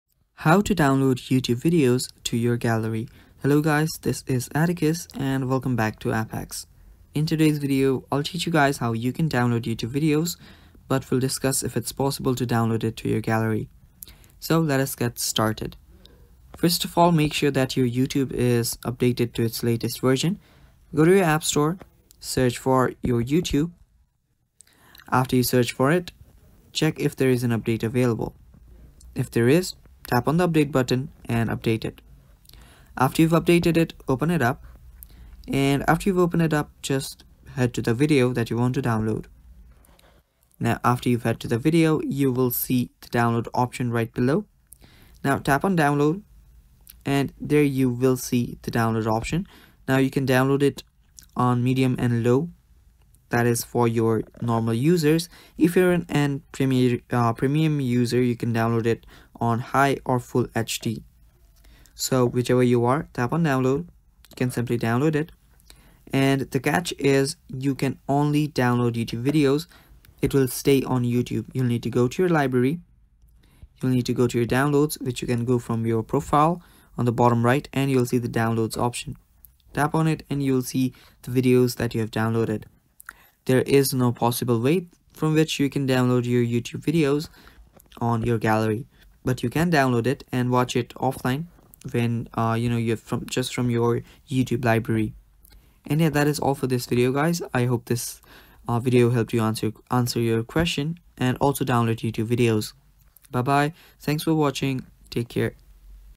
How To Download YouTube Videos To Gallery
Learn how to easily download YouTube videos directly to your phone’s gallery in this step-by-step tutorial. Whether you're looking to save videos for offline viewing or organize your collection, this guide will show you how to download YouTube videos without using any third-party apps!

In this video, you'll discover the simplest methods for downloading YouTube videos to your gallery, including tips for downloading videos on both Android and iPhone devices.

Timestamps:
0:00 Introduction
0:20 Step 1: Finding the Video You Want to Download
0:45 Step 2: Downloading the Video to Your Gallery